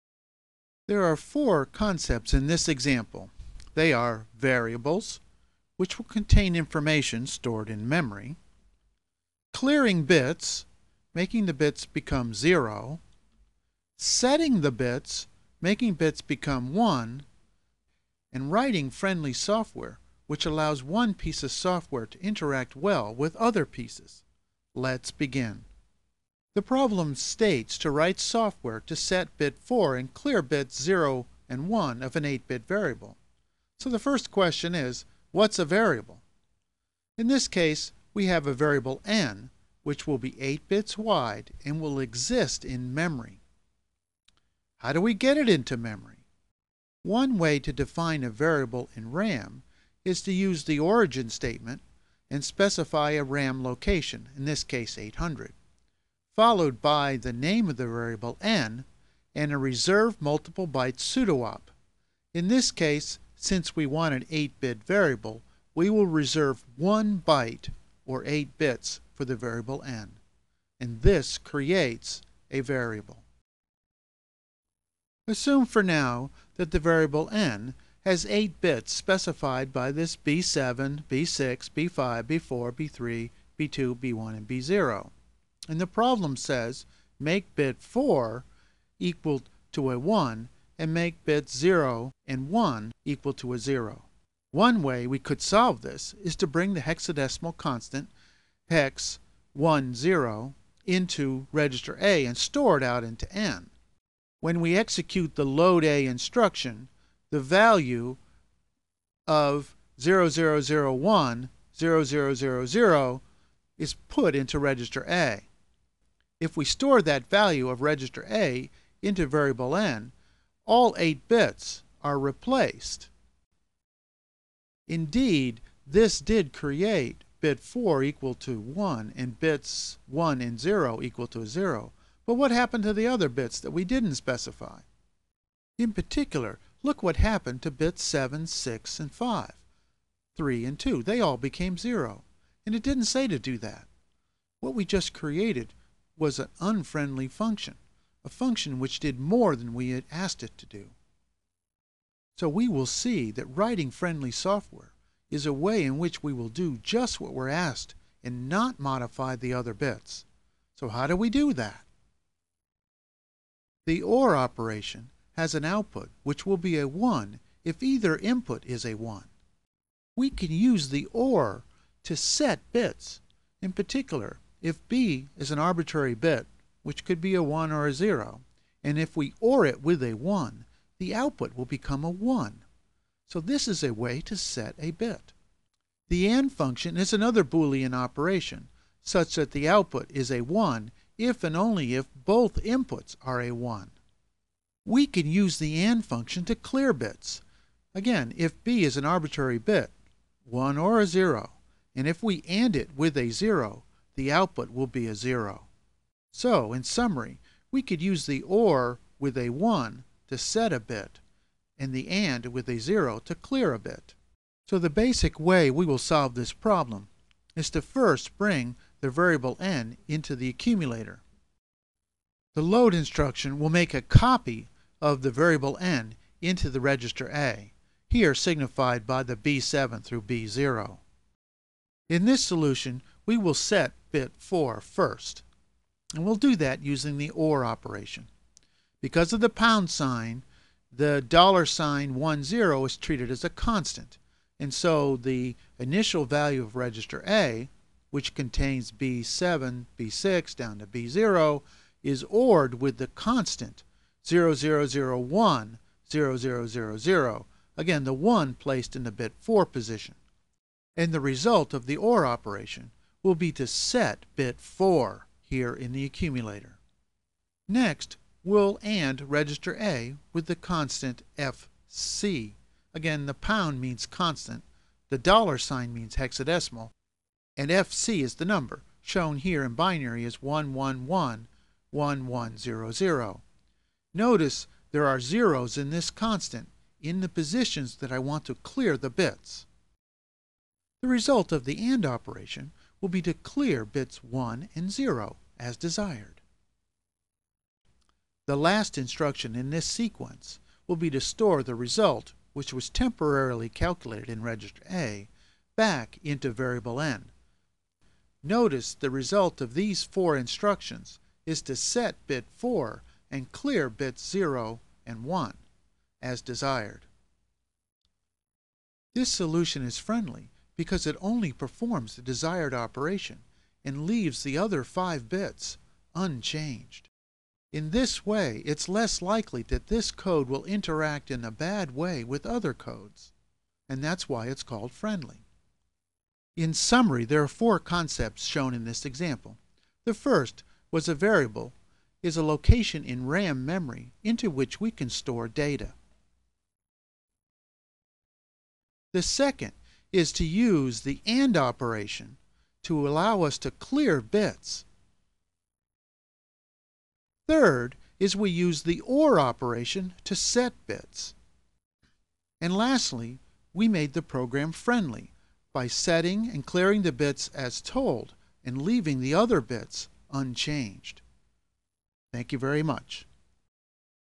Example 3.1.
Write software to set bit 4 and clear bits 1 and 0 of an 8-bit variable N.
Introduction to Embedded Systems: Interfacing to the Freescale 9S12
Cengage 2009, ISBN-10: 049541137X   ISBN-13: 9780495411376
University of Texas at Austin